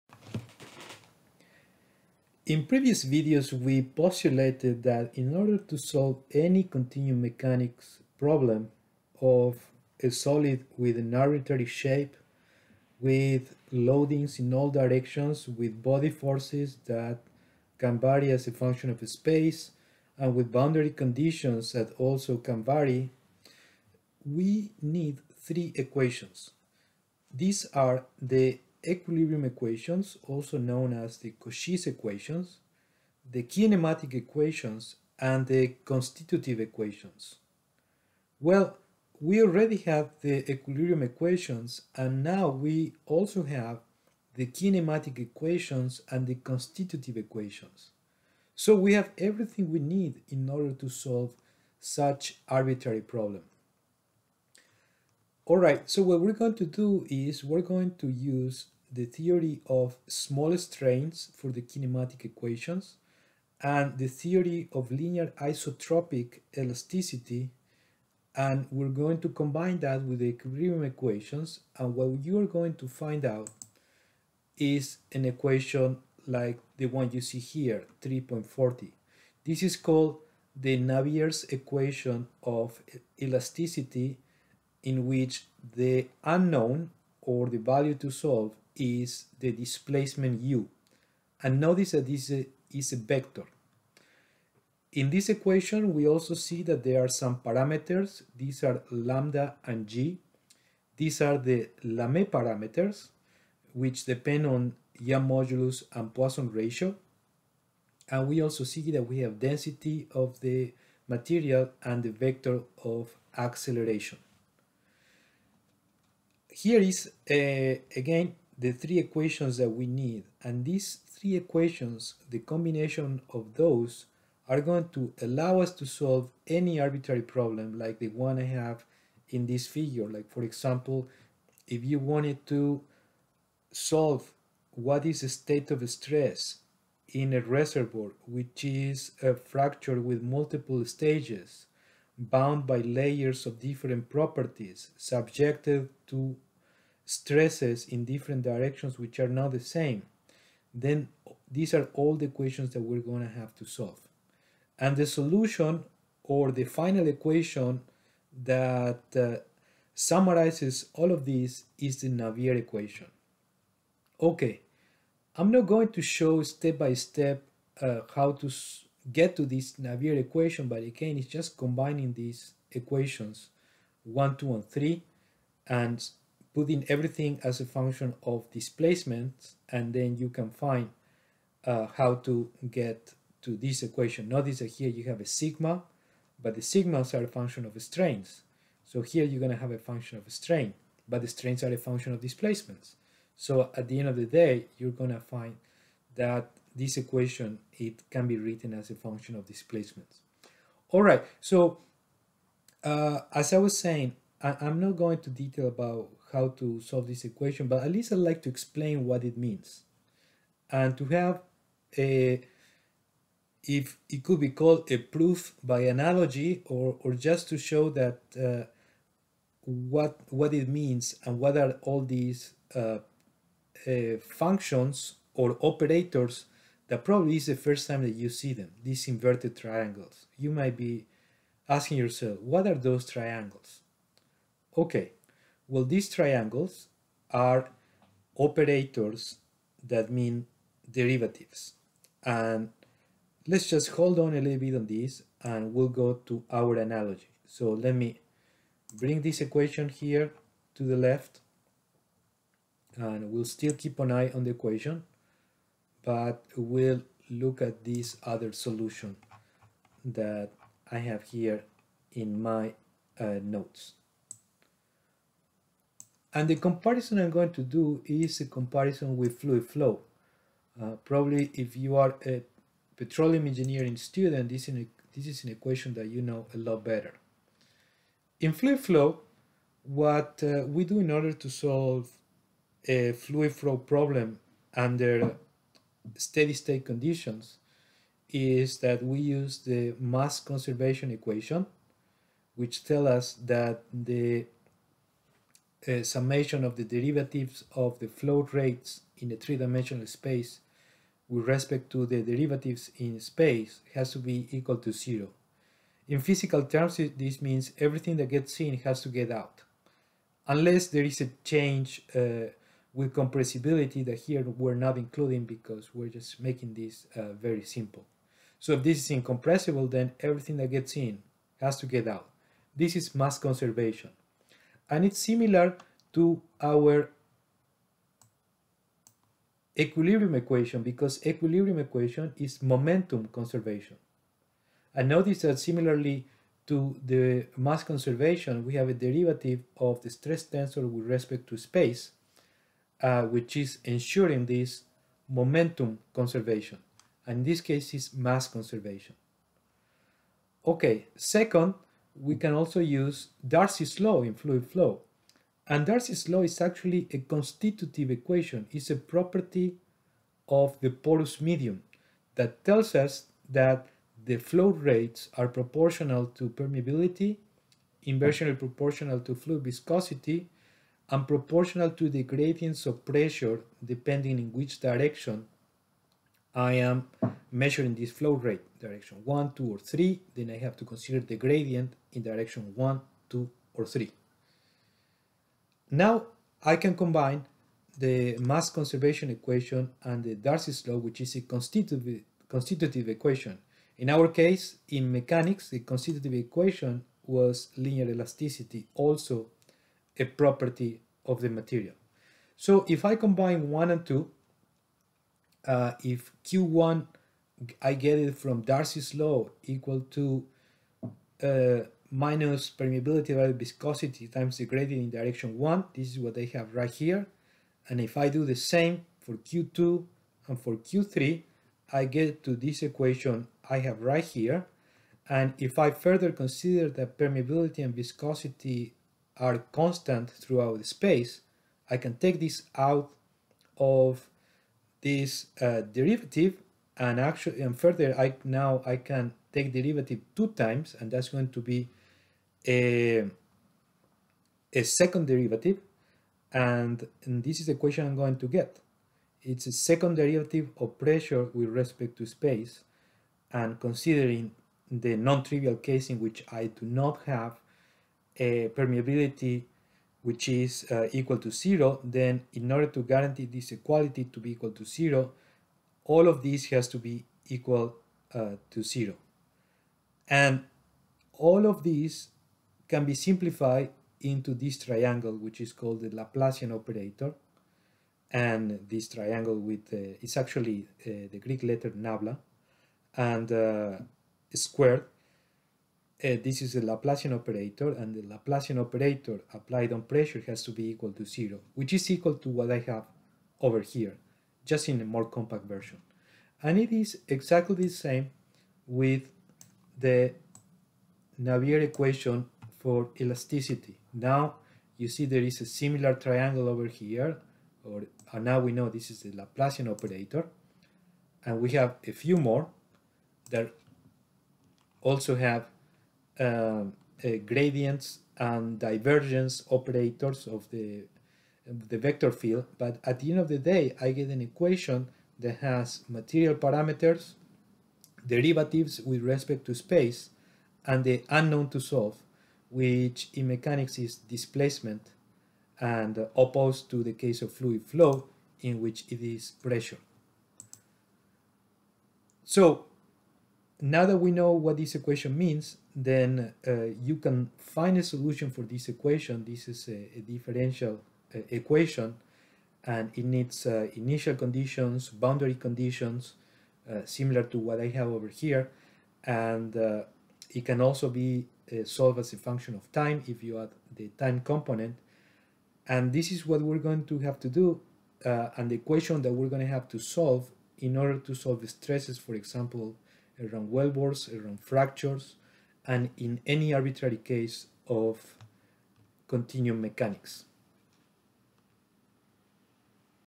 L11 Navier's linear isotropic elasticity equation
This is a video recording of Lecture 11 of PGE 334 Reservoir Geomechanics (Fall 2020) recorded on September 21, 2020, at The University of Texas at Austin by D. Nicolas Espinoza

Topics covered: general solution of a linear isotropic elasticity problem, analogy with fluid flow equations.